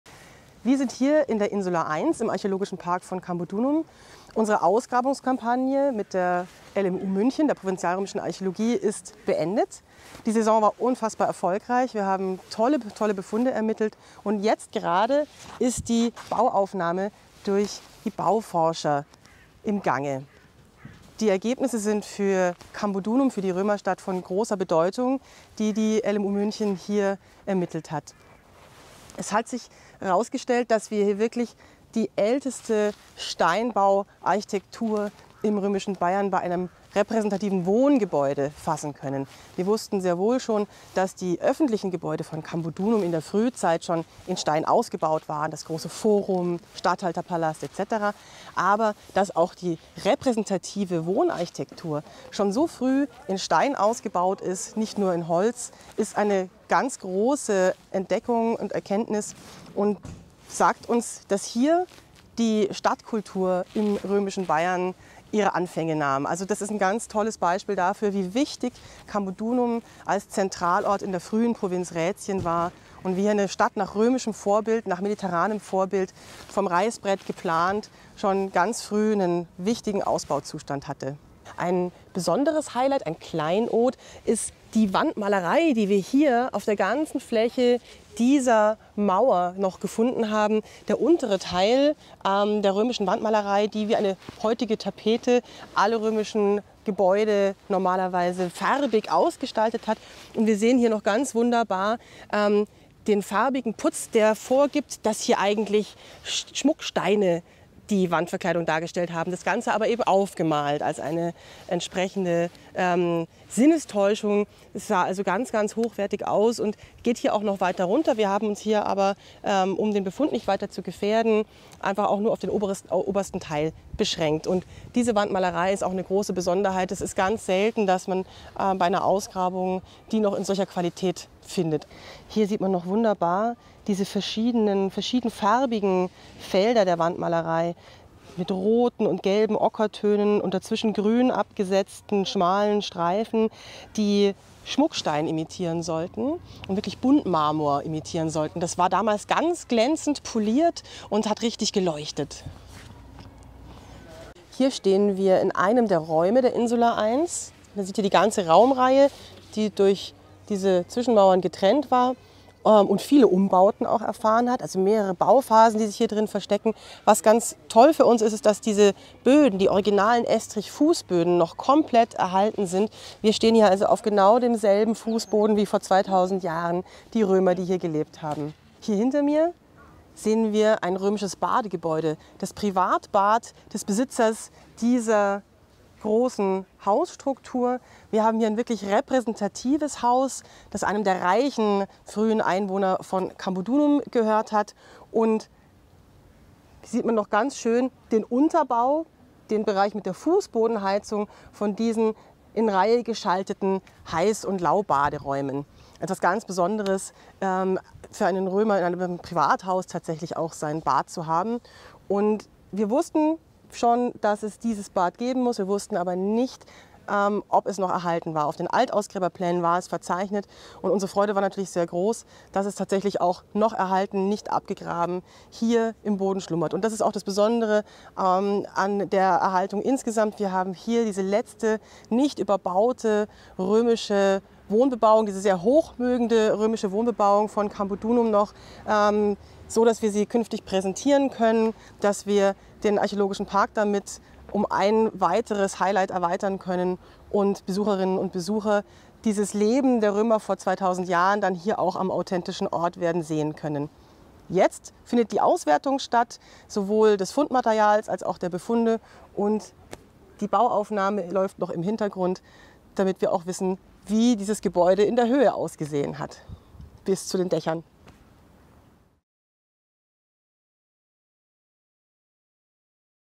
Dr. Maike Sieler präsentiert Ergebnisse der Grabung im Archäologischen Park Cambodunum 2022
Die Stadt Kempten als älteste schriftlich erwähnte römische Stadt Deutschlands widmet sich seit 2019 in einem Kooperationsprojekt mit der Ludwig-Maximilians-Universität München und dem Bayerischen Landesamt für Denkmalpflege der Erforschung der Römerstadt Cambodunum.

Ein süddeutschlandweit einzigartiger Befund im Archäologischen Park Cambodunum – Die bei Ausgrabungen zutage gekommenen römischen Steinbauten gehören zu den ältesten in Deutschland.

Archäolog:innen haben in Kempten ein repräsentatives Wohnhaus mit intakten Estrichböden, einer privaten Thermenanlage sowie Wandmalereien ausgegraben und zeigen die zentrale Bedeutung von Cambodunum als römische Zivilsiedlung in Bayern auf.

Dr. Maike Sieler, Leiterin des Archäologischen Parks Cambodunum und Stadtarchäologin stellt die großartigen Ergebnisse vor.